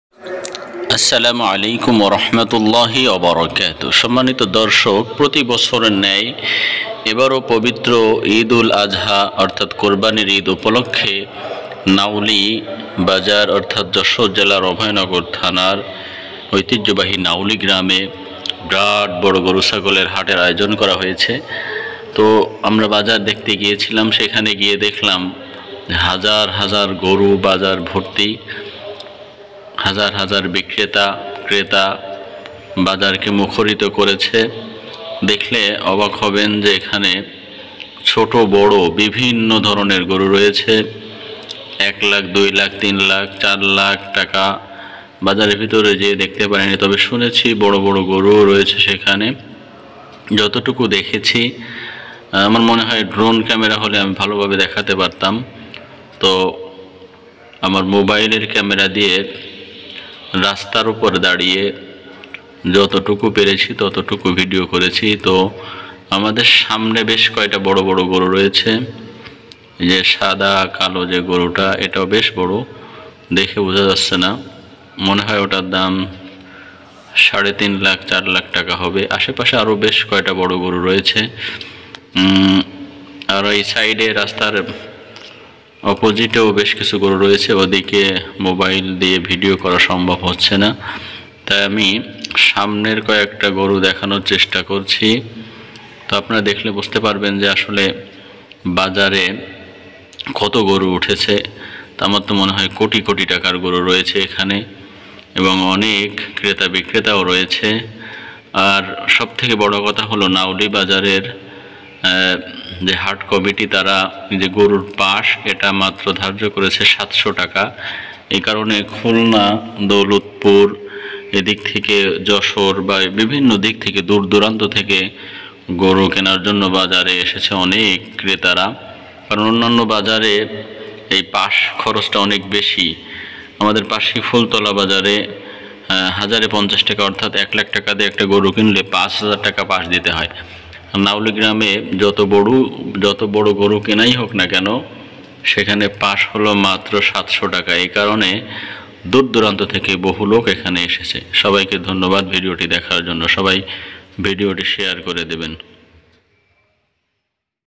গরুর দাম কত নিলো ভাই || কুরবানীর পশুর দাম কেমন || ঈদে কুরবানীর পশুর দাম/দর
ঈদে কুরবানীর পশুর হাট জমে উঠেছে, বিরাট গরু ছাগলের হাট, গরুর দাম কত নিলো , পশুর দাম দর, বড় বড় গরু ছাগল, যশোর অভয়নগরের ঐতিহ্যবাহী নাউলী গ্রাম।

Description
652-91
gabtoli_gorur_haat_2023
cow price 202311 Hazrotpur haat 2023